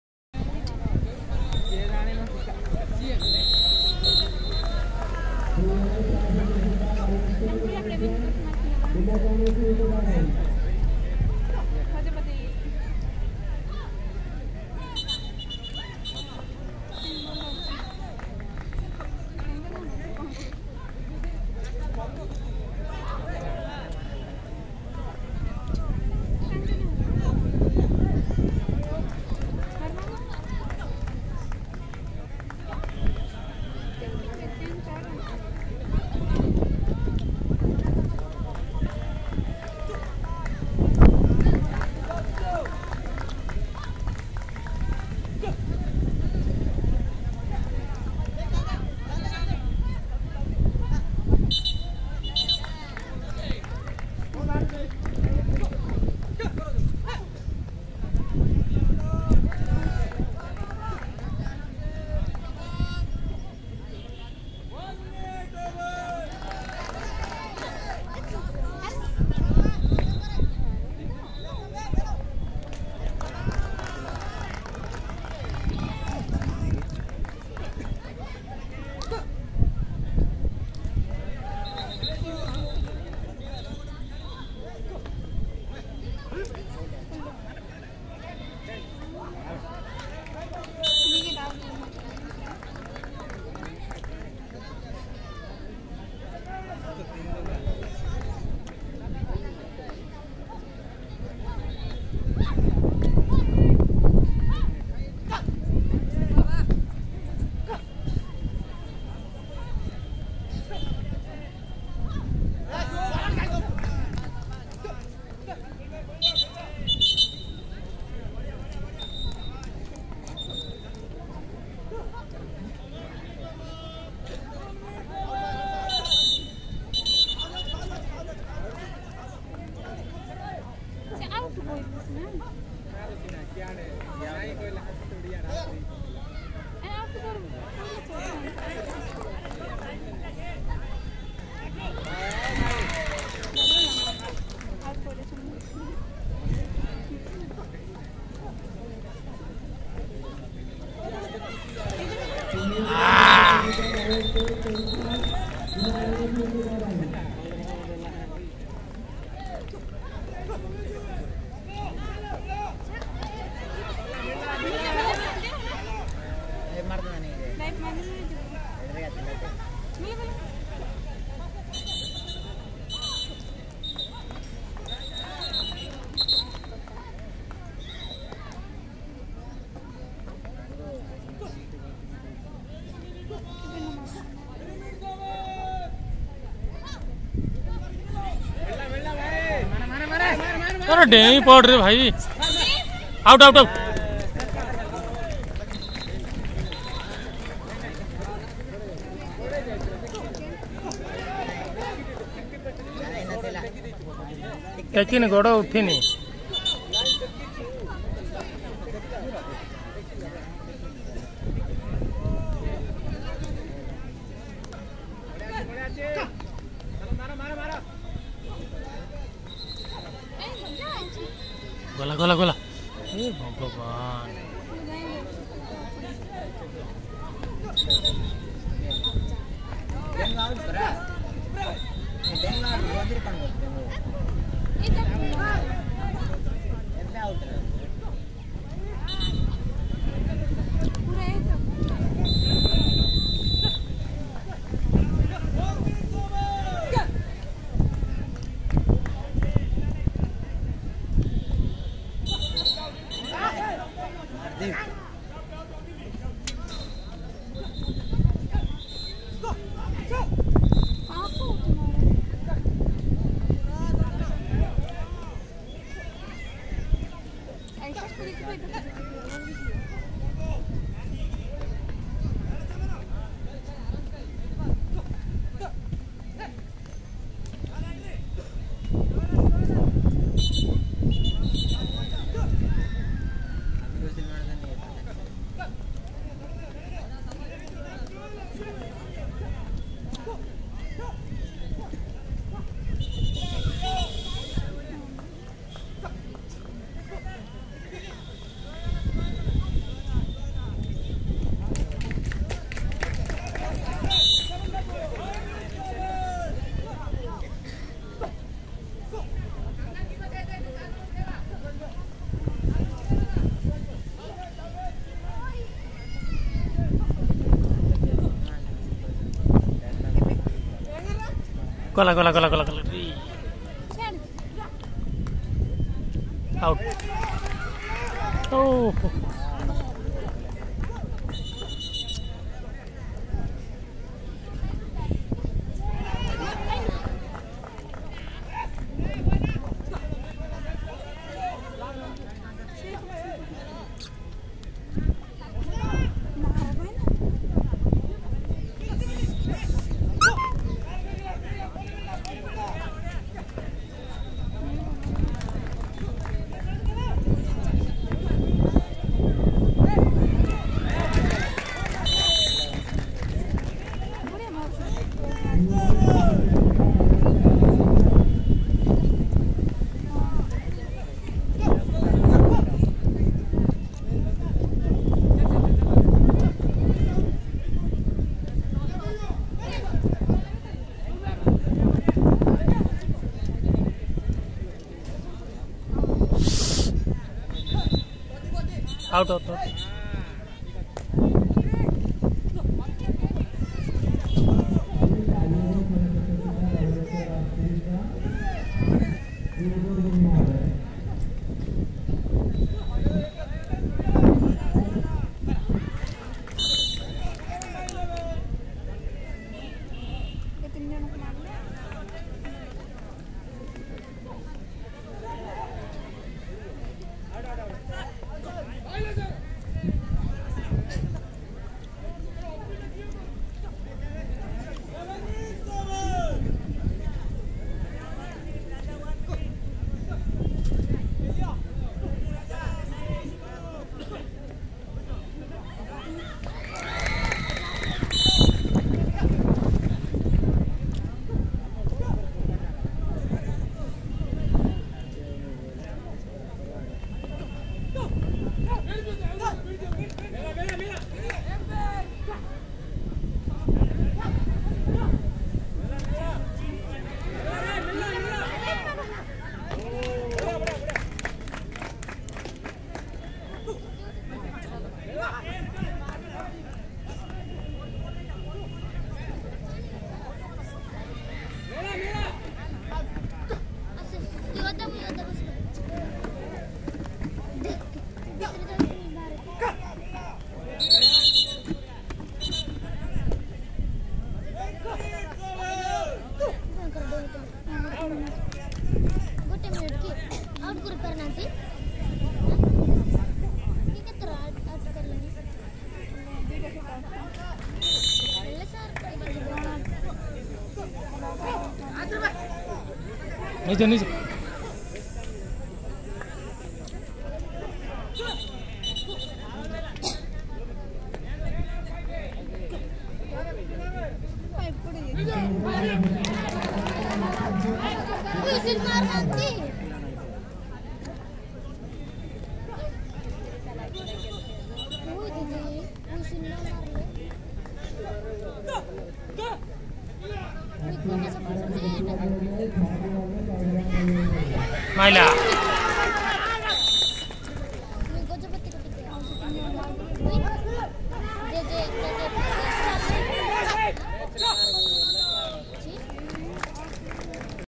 CUTTACK 🆚 GAJPATI KHO KHO MATCH 🧐🏟️ PART 2
YUVA ODISHA NAVEEN ODISHA INTER DISTRICT KHO KHO CHAMPIONSHIP AT PURI 2024